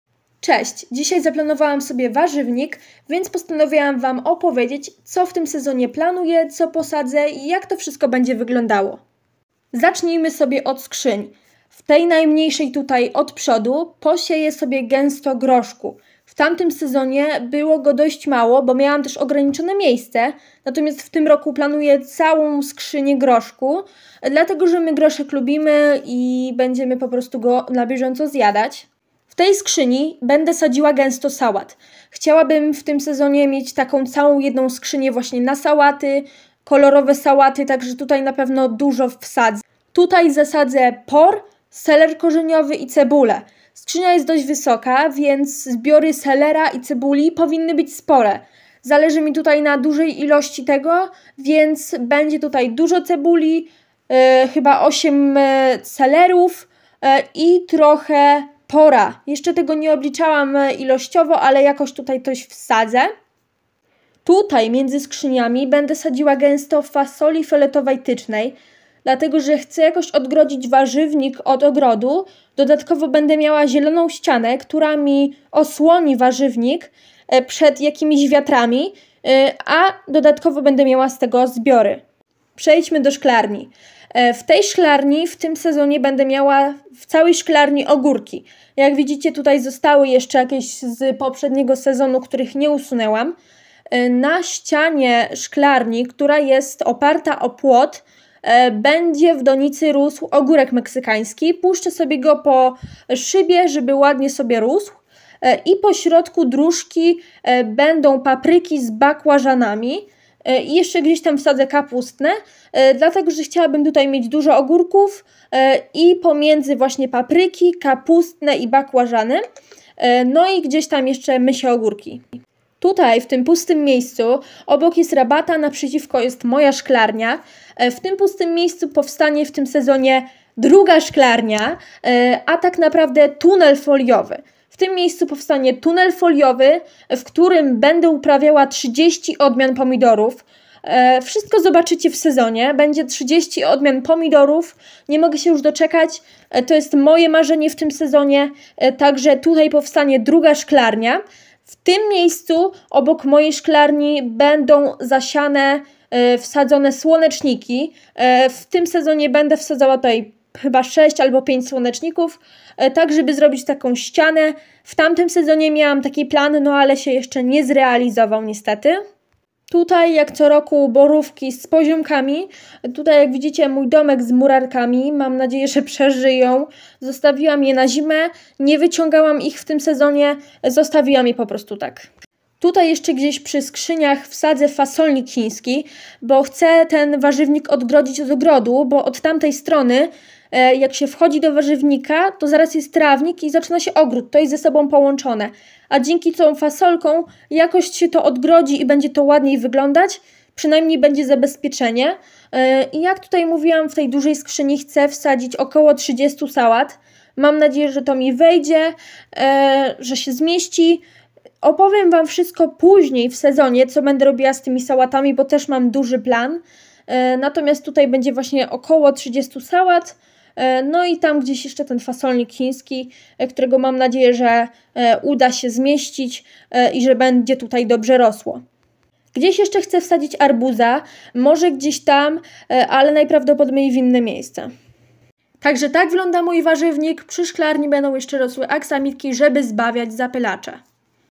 PLANY NA SEZON 2223, CO GDZIE POSADZĘ, MOJE NOWE MARZENIE NA TEN SEZON. \ NATKA SIEJE
Cześć witajcie dzisiaj opowiem wam o moich planach na ten sezon, co gdzie i jak posadzę.
Opowiem wam o moim tegorocznym marzeniu ogrodniczym.